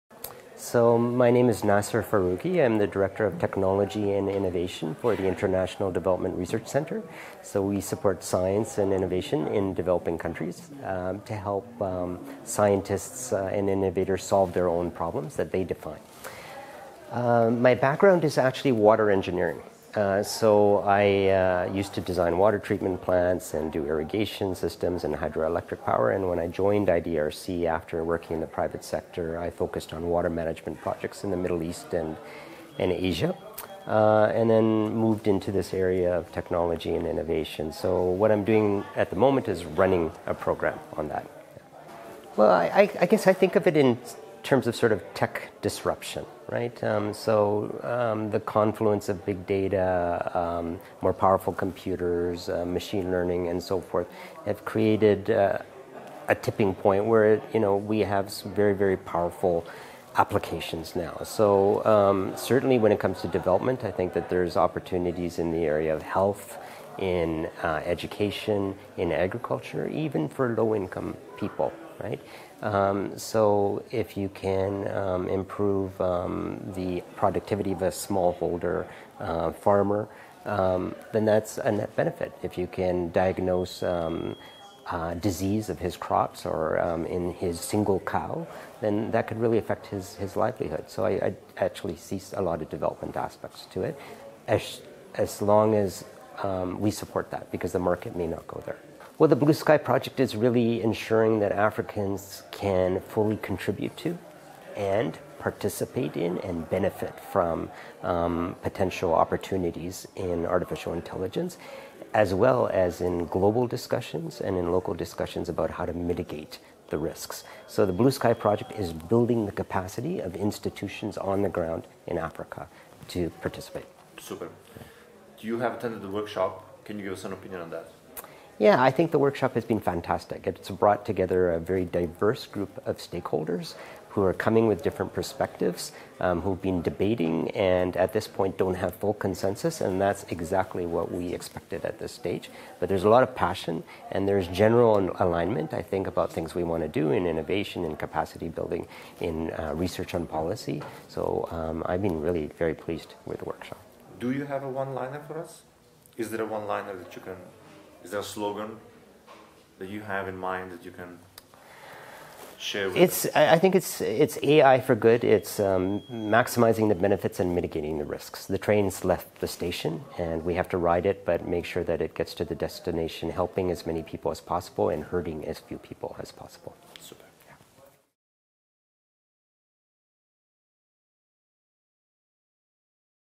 Naser Faruqui, IDRC discussing AI and its impact for development
AI4D Interviews Series
Follow us @AI4Dev
Speaker: Naser Faruqui, International Development Research Centre (IDRC)

This interview was part of a series filmed at the kick-off of the new Network of
Excellence in Artificial Intelligence for Development in Nairobi, Kenya and is part of
the efforts of the AI4D initiative to continue this momentum and to support
Africa’s emerging AI ecosystem. The emerging network of machine learning and AI
practitioners and researchers undertaking a collaborative roadmap for AI for
Development in Africa. The three-day workshop zoomed in on three critical areas of
1) policy and regulations, 2) skills and capacity building and 3) the application of AI
in Africa